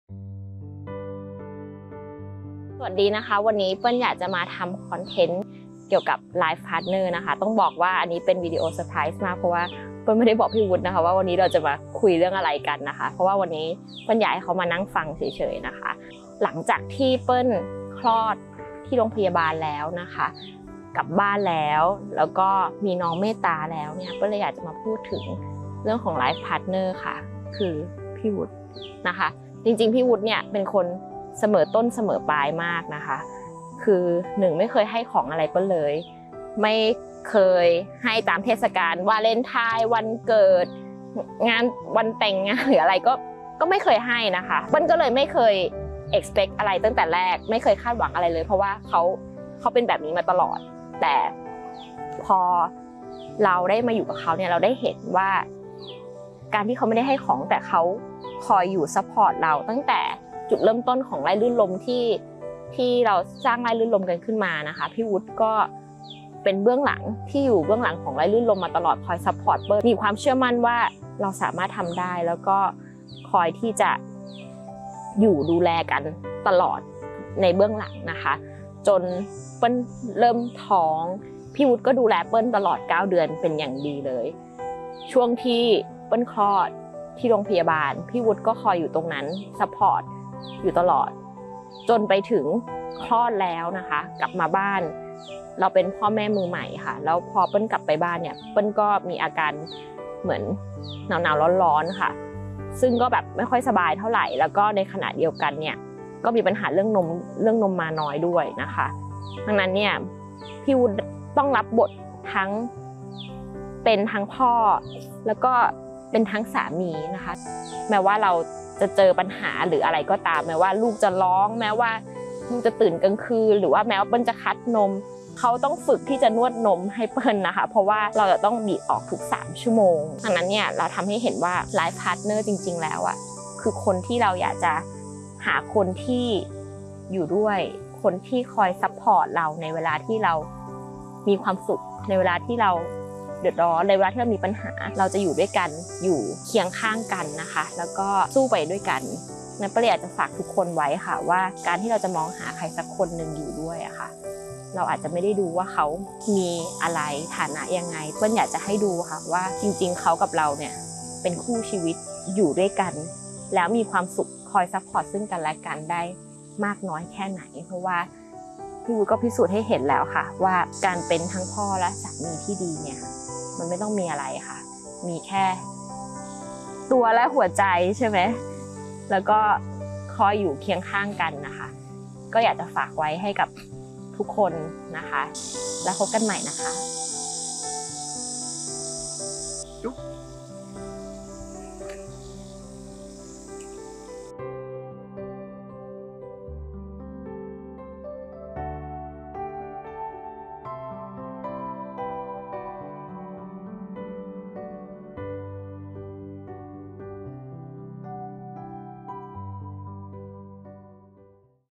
Life Partner นิยามคำว่า "ครอบครัว" ในแบบฉบับของเปิ้ล
Meaningful of "Life Partner"
มีหลายคนที่ผ่านเข้ามาในชีวิตของเปิ้ล
ไม่ว่าจะเป็นครอบครัว เพื่อน ญาติพี่น้อง

แต่ถ้าพูดถึงเรื่องของ Life Partner...
บางทีเราอาจมองข้ามคนที่ธรรมดา ที่อยู่ข้างๆเรา
คนที่คอยเป็นกำลังใจในทุกๆช่วงเวลาของชีวิต

ในวันนี้เปิ้ลเลยอยากจะขอบคุณ Life Partner ของเปิ้ล
คือพี่วุฒิ เป็นคนที่เสมอต้นเสมอปลายมาก
ไม่เคยซื้อของให้ตั้งแต่เริ่มต้น ไม่เคยได้อะไรในทุกเทศกาล
เปิ้ลเลยไม่คาดหวัง แต่พี่วุฒิก็พิสูจน์ให้เห็น
ตั้งแต่แรกตั้งแต่ที่เริ่มสร้างฟาร์มไร่รื่นรมย์มาด้วยกัน🌿🌿
เป็นคนที่เบื้องหลังคอย support เปิ้ลมาตั้งแต่เริ่มต้น
จนมามาตอนนี้มีลูกด้วยกันก็ดูแลเปิ้ล
ตั้งแต่ตั้งครรภ์จนคลอดมาเป็นอย่างดี

แต่มันยังยังไม่จบเท่านี้...
เพราะหลังจากคลอดเปิ้ลก็มีปัญหา
เรื่องน้ำนมที่ผลิตได้แต่ออกยาก
ตอนอยู่รพ.ก็ยังมีพยาบาลคอยช่วยนวด
แต่พอกลับบ้าน ก็ต้องเป็นหน้าที่สามีที่คอยนวดนมให้

พี่วุฒิต้องรับบทหนัก 2 เท่าเพราะต้องให้นมลูกด้วย
เพราะเปิ้ลไม่สบาย และนวดนมเปิ้ลทุกๆ 3 ชม.

ด้วยเหตุการณ์ต่างๆเหล่านี้ทำให้เปิ้ลเห็นว่า...
เปิ้ลไม่อยากได้ของอะไร อยากได้คนที่เขาเข้าใจและดูแลเราได้
และพร้อมจะสู้และเผชิญไปกับทุกปัญหาที่เข้ามาด้วยกัน
รู้สึกซาบซึ้งใจ และดีใจที่ได้เขามาเป็นสามีและเป็นพ่อของน้องเมตตา

ไร่รื่นรมย์ ฟาร์มเกษตรอินทรีย์
Rai Ruen Rom Organic Farm